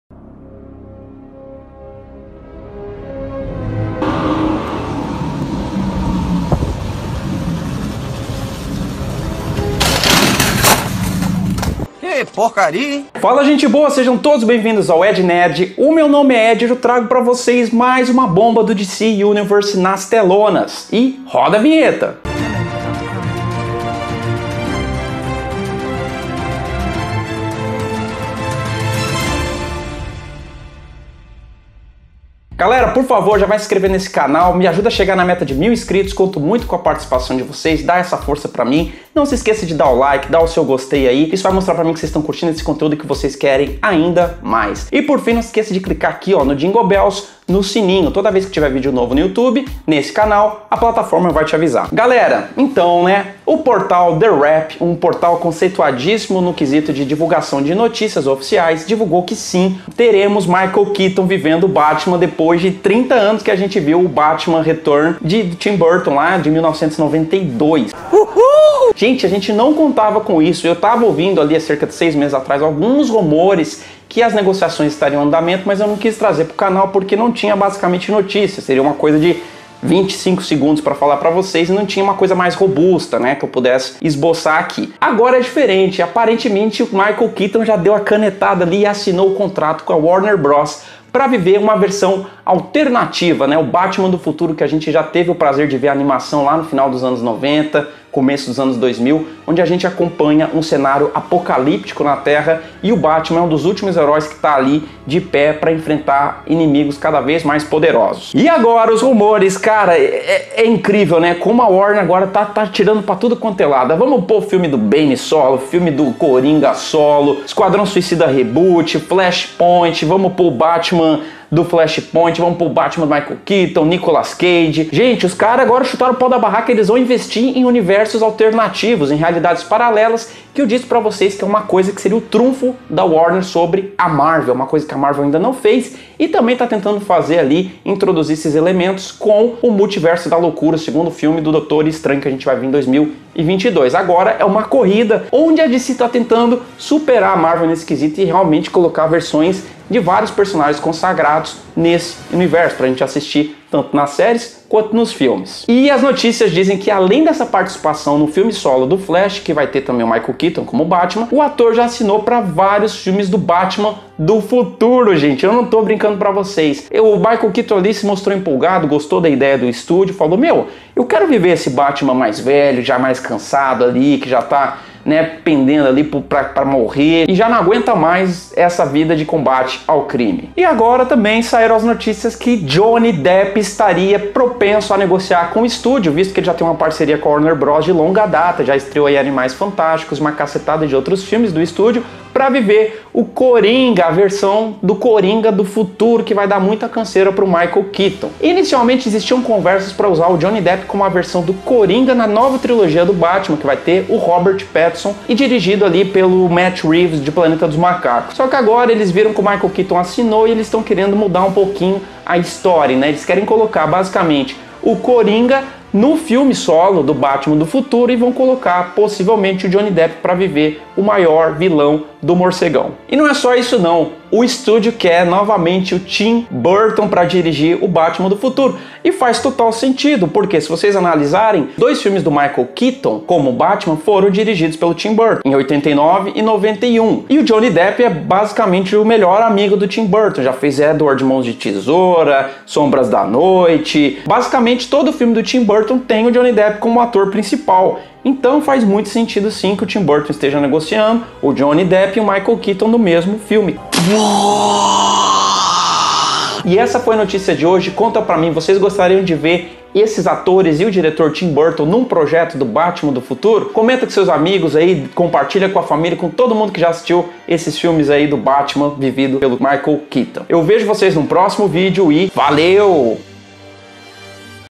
FILME DO BATMAN DO FUTURO E JOHNNY DEPP COMO CORINGA!!!
Fala gente boa, blz? Hoje eu trago para vocês uma notícia bem louca sobre  um filme que a Warner está planejando que pode ter Michael Keaton como Batman e Johnny Depp como Coringa!
Valeu!!!